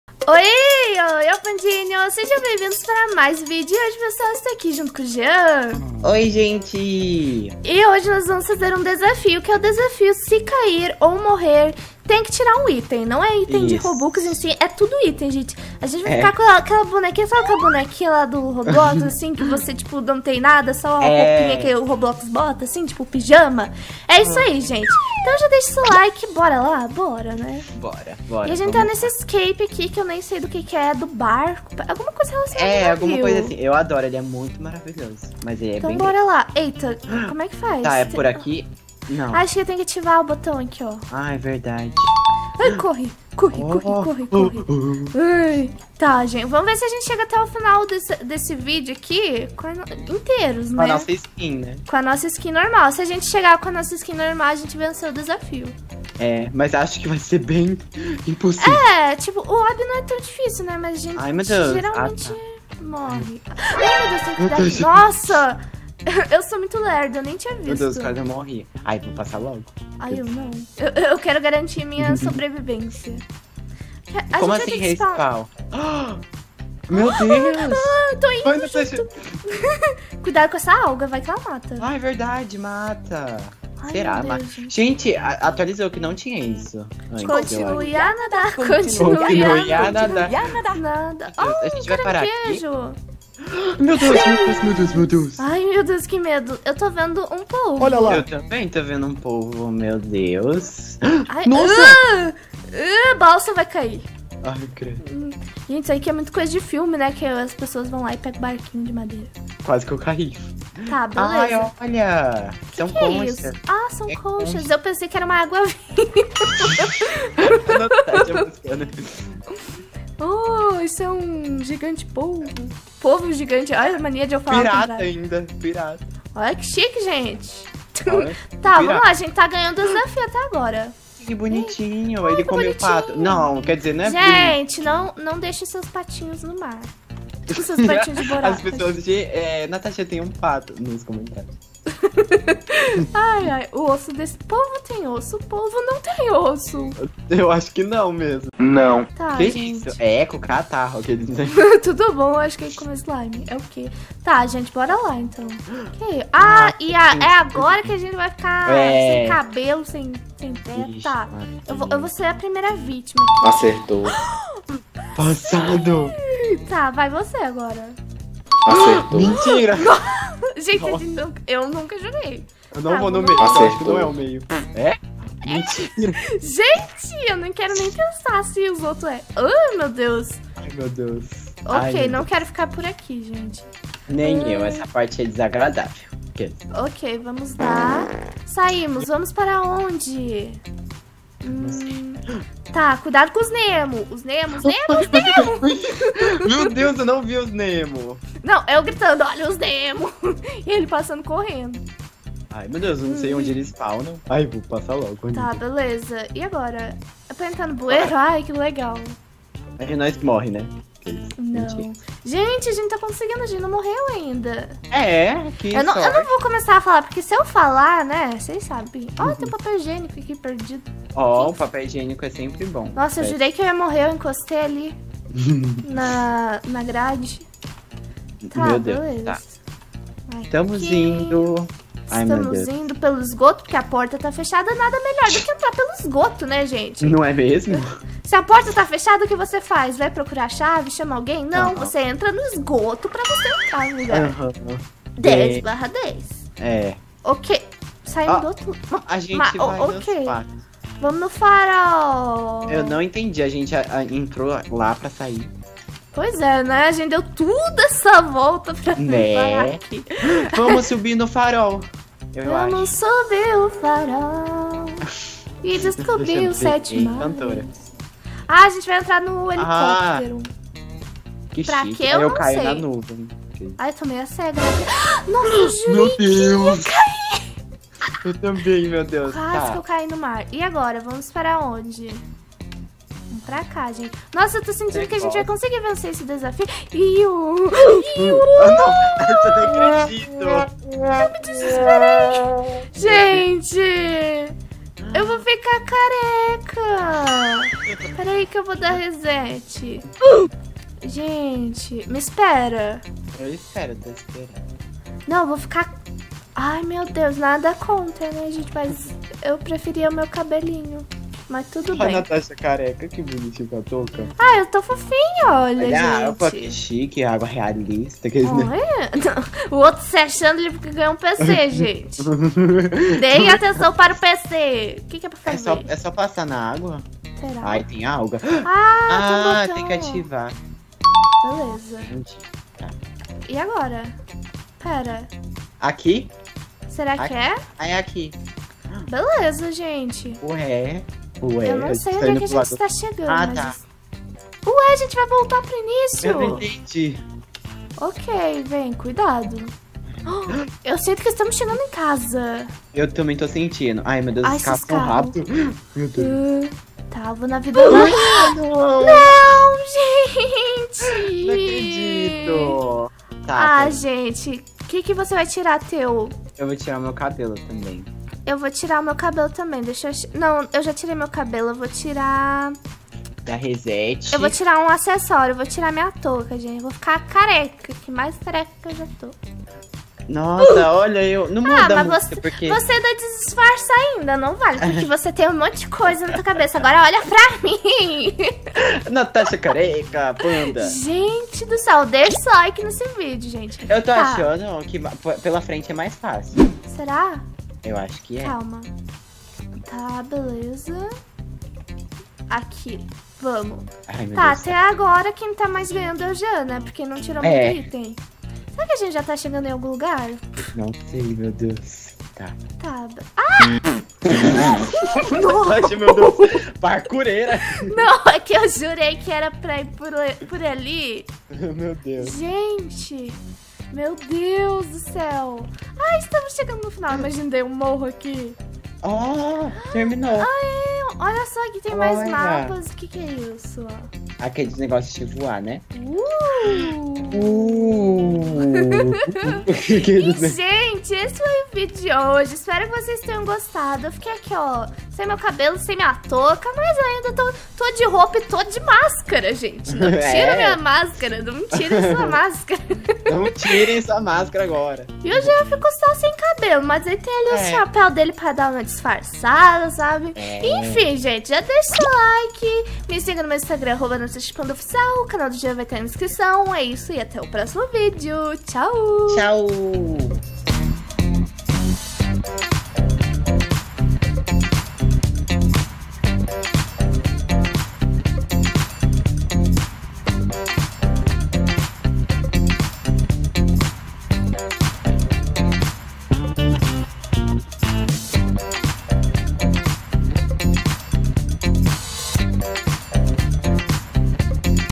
SE CAIR OU MORRER TIRA UM ITEM DO AVATAR! 😂
🐼Pessoal hoje eu e o jeancof fizemos o desafio ''Se cair ou morrer tem que tirar um item do avatar'' será que chegamos ao final desse parkour inteiros?? 😂 Assista até o final pois está super legal :D

NatashaPanda